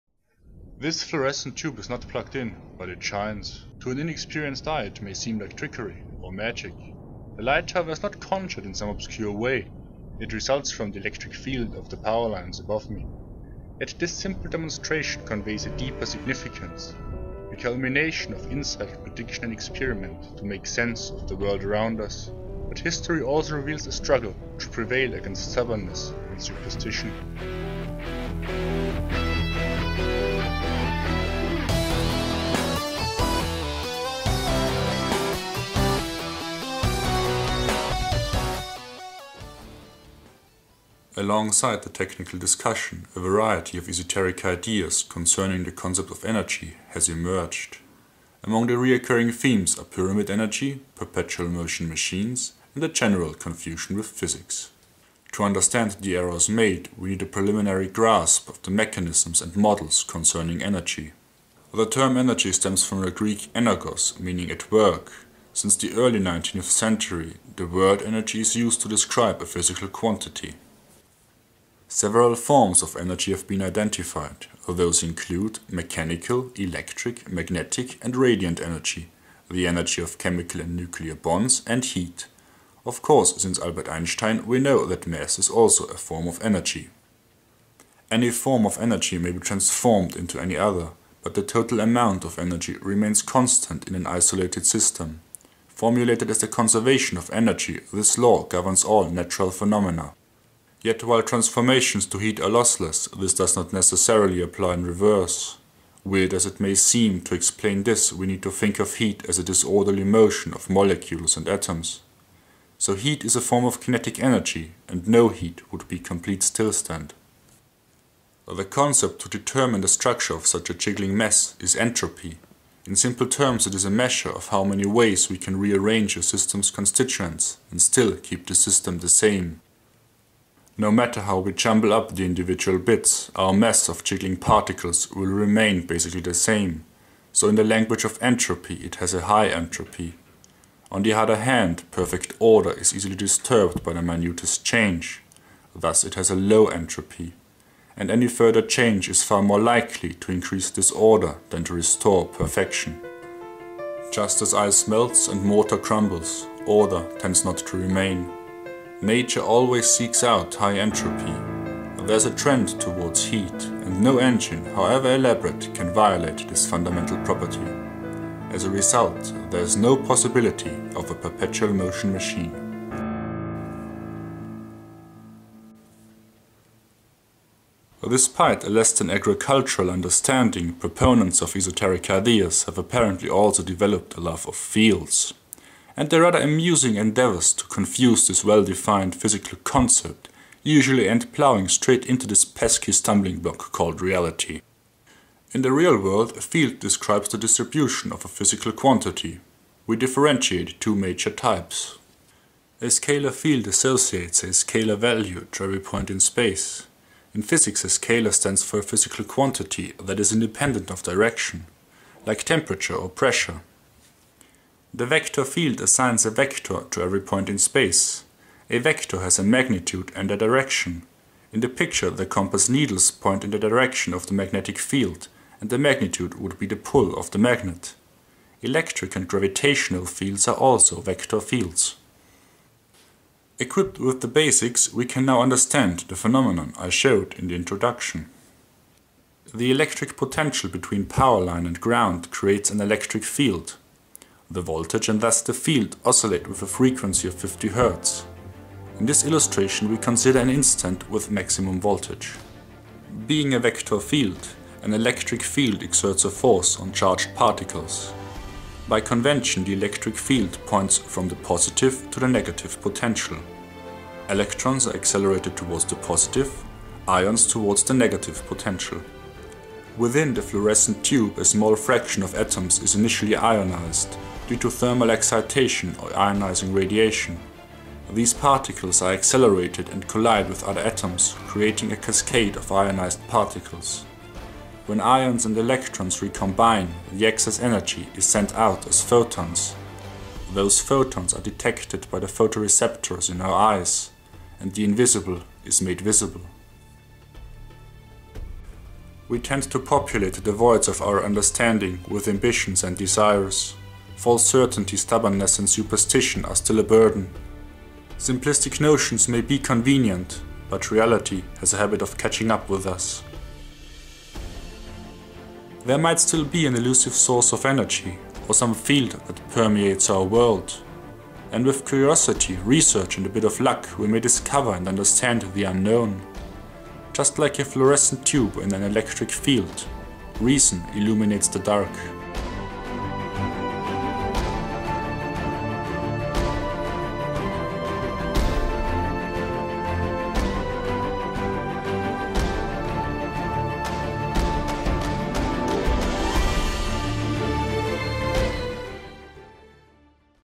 Energy: A Call for Reason
Rather than debunking the misconceptions of free energy proponents (has been done quite often), I try to present a general introduction to the physics concerning energy. This however is a simplified version and for further information you should look up the corresponding wikipedia articles or my sources. Of course you are invited to comment and I will try and answer questions or clarify if needed.

I use the term free energy to refer to bogus claims concerning energy. Legit forms of free energy (sunlight, wind) are thus excluded from the definition in this video.

(5) Experimental-Physik 2, Prof. Dr. Jäger, 34th edition, February 2012